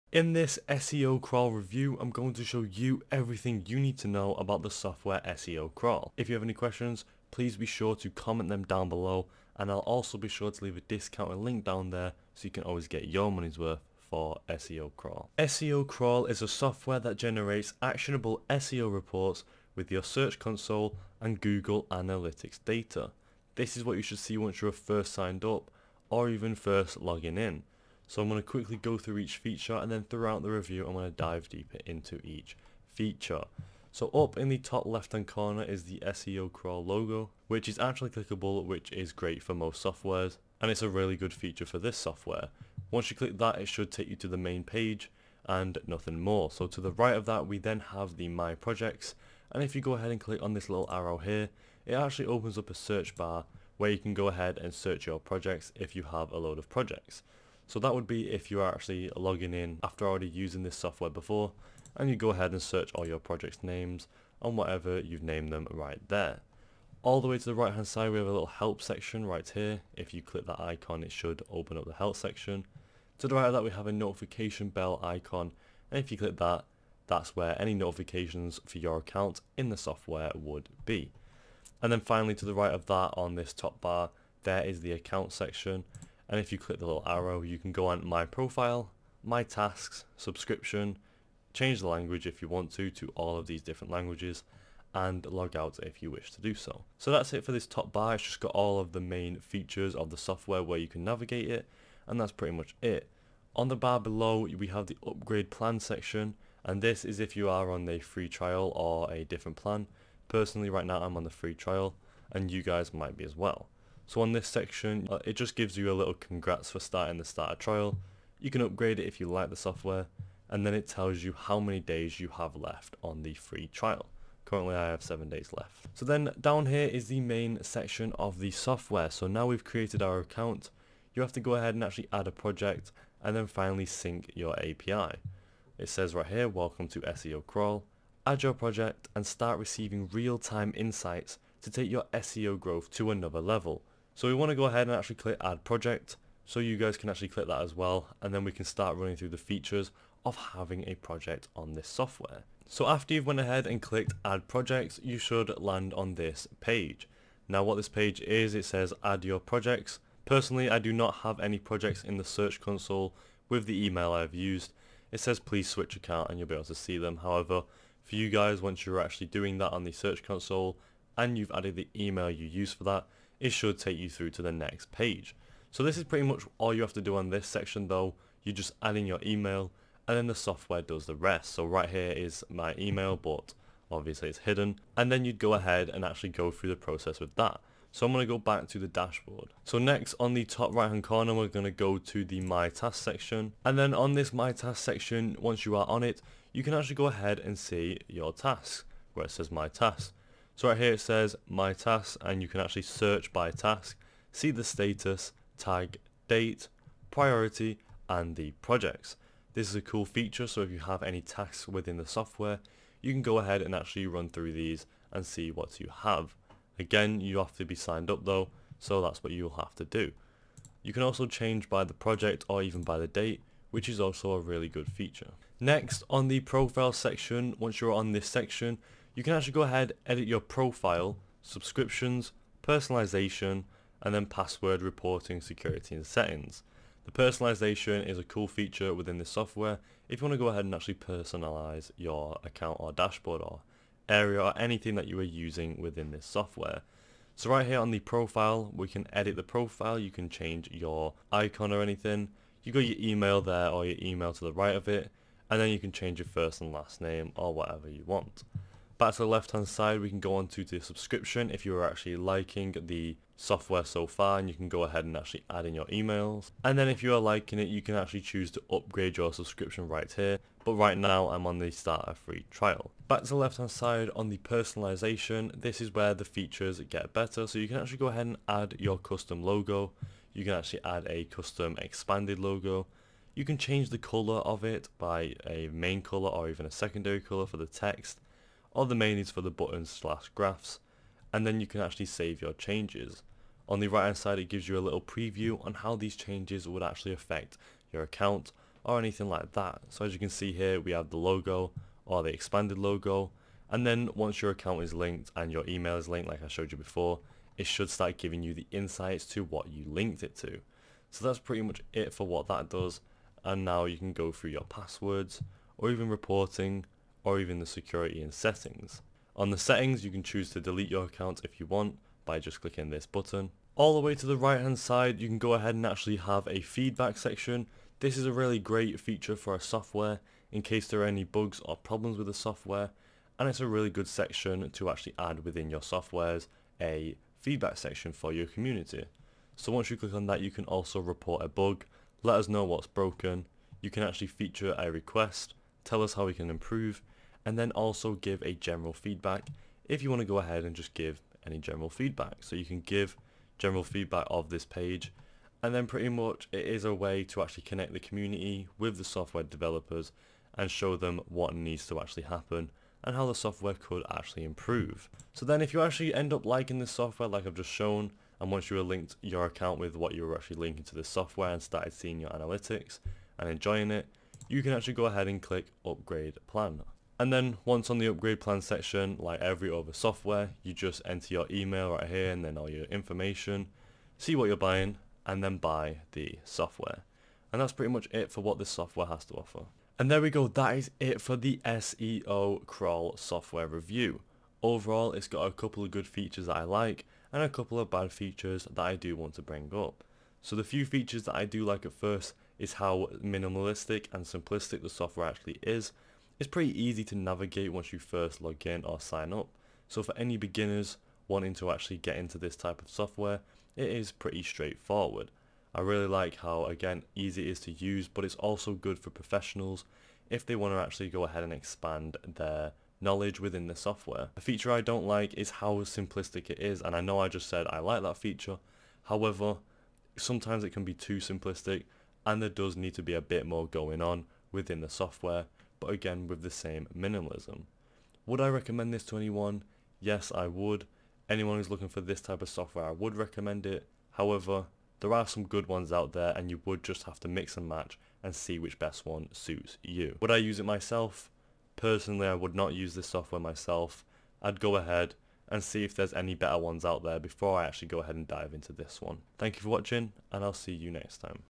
Seocrawl Review: The Truth About Seocrawl
Seocrawl Review: Ready to see the power of Seocrawl? I'll be showcasing all the things you want to discover about Seocrawl in this Seocrawl review!

In this Seocrawl review Ill be answering the following questions:

1. What is Seocrawl and what does it do
2. How does Seocrawl work
3. The pros and cons of using Seocrawl
4. Who should use Seocrawl?
5. How to get started with Seocrawl

🕐Chapters🕐
0:00 Seocrawl Review
0:10 Seocrawl Discount
0:20 Exploring Seocrawl Dashboard
0:40 What is Seocrawl? Answered
01:50 Pro's and Con's of Seocrawl
06:00 Getting started with Seocrawl
06:30 Who is Seocrawl for?